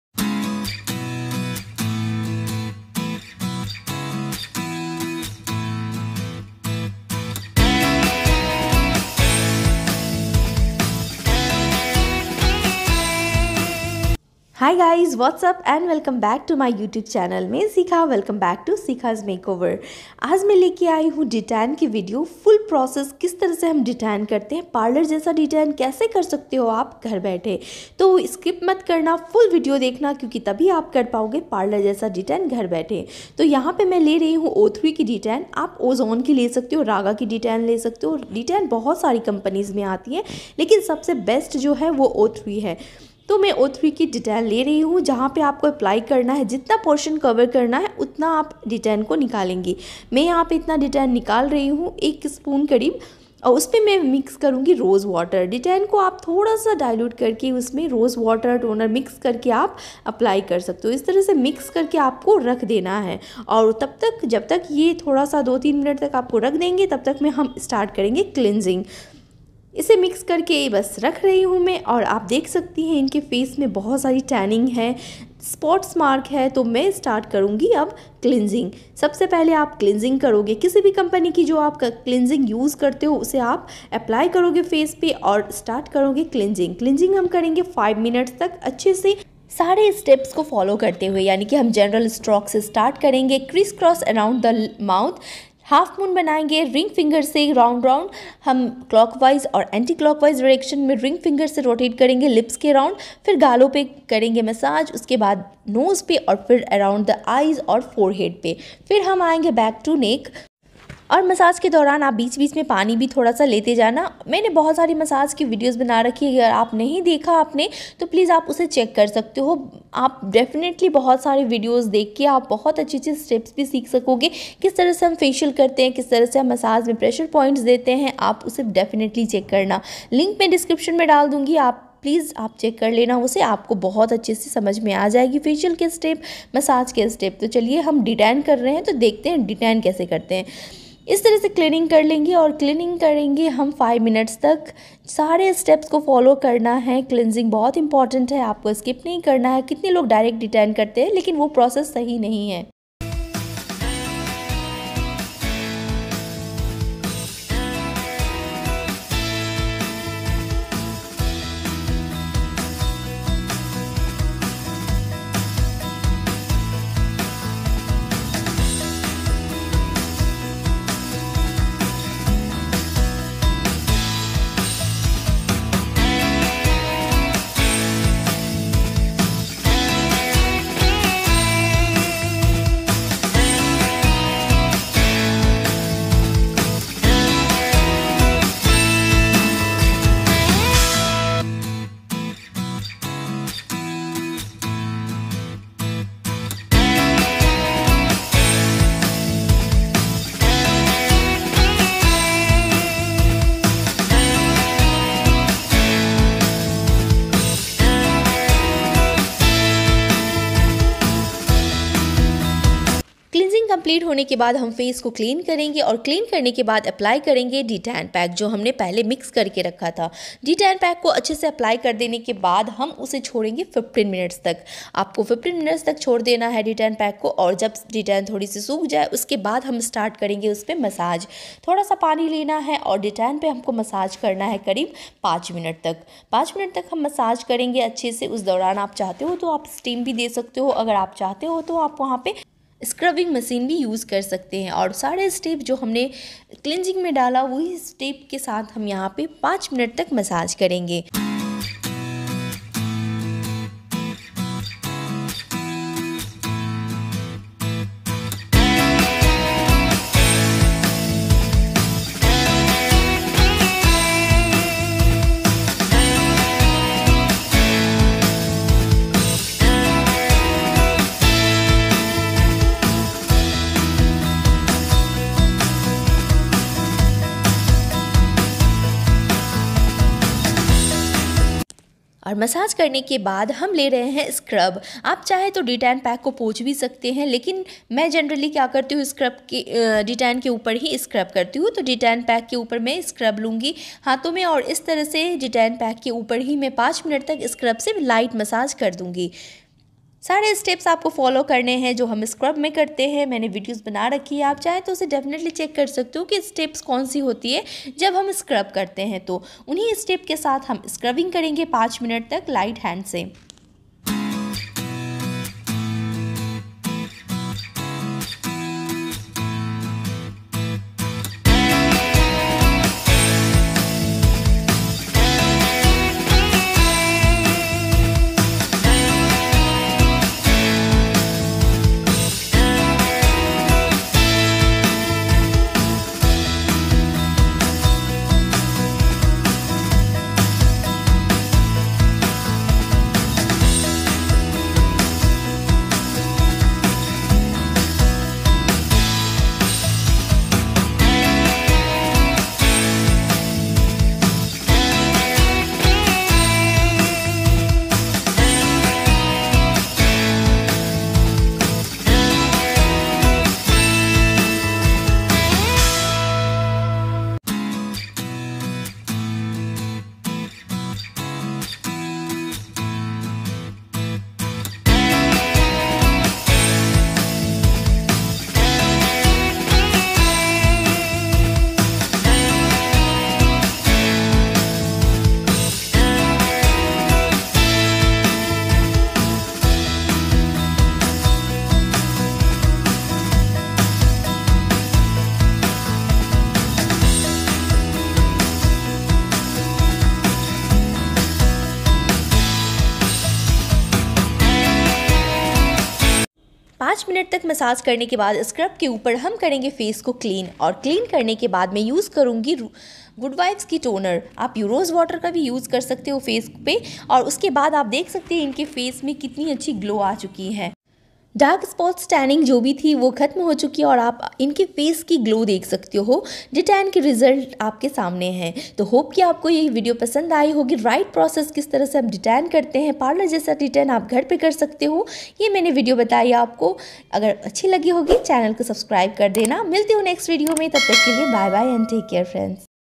D-tan क्याहै/सिखेparlourजैसा D-Tanकरना घर पे🔥/D- tanकरनेका सही तरीका/full process@Sikha's Makeover
dtan kaise karte hai /dtan karne ka sahi tarika /dtan kare at home /parlour jaise D tan ghar pe